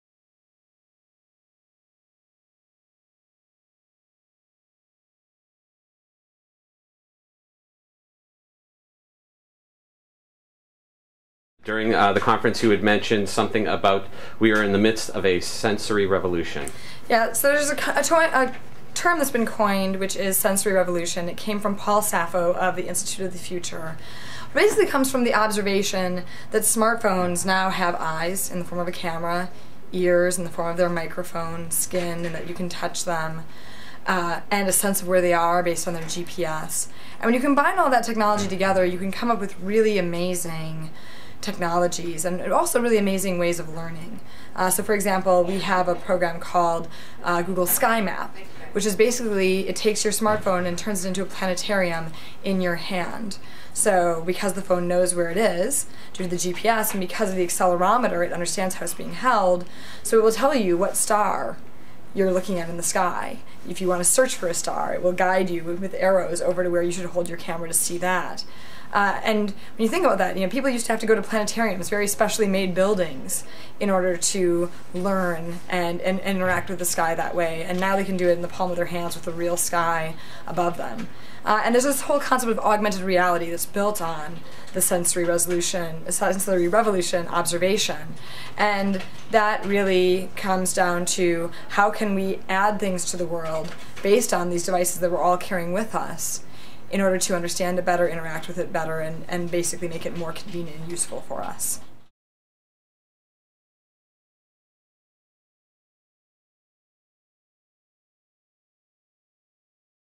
Marissa Mayer @ Breakthrough Learning Forum - Sensory Revolution
Marissa Mayer is the Vice President of Search Products and User Experience for Google. Since she joined Google, she has been responsible for more than 100 features and products made available on Google.com. She has also been awarded several patents for her work in artificial intelligence and interface design.

During Marissa's presentation at the Breakthrough Learning event, she mentioned how we are all currently in the midst of a "Sensory Revolution". In this clip, Marissa discusses what she means by the term Sensory Revolution and its relationship to augmented reality, citing the work of Paul Saffo